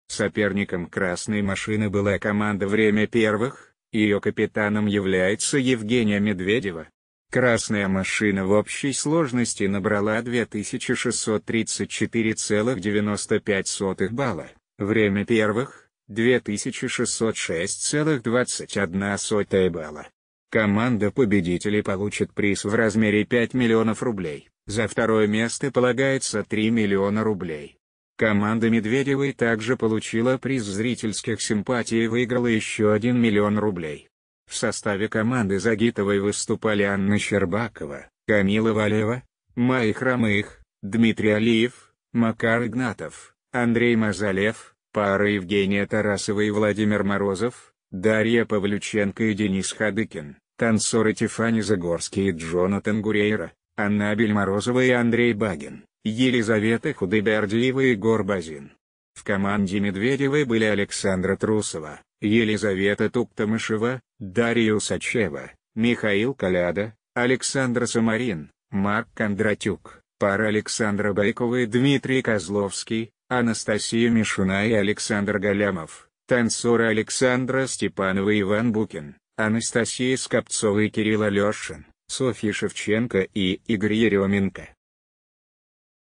Команда Загитовой выиграла Кубок Первого канала по фигурному катанию
Соперником «Красной машины» была команда «Время первых», ее капитаном является Евгения Медведева. «Красная машина» в общей сложности набрала 2634,95 балла, «Время первых» — 2606,21 балла. Команда победителей получит приз в размере 5 млн рублей, за второе место полагается 3 млн рублей. Команда Медведевой также получила приз зрительских симпатий и выиграла еще 1 млн рублей.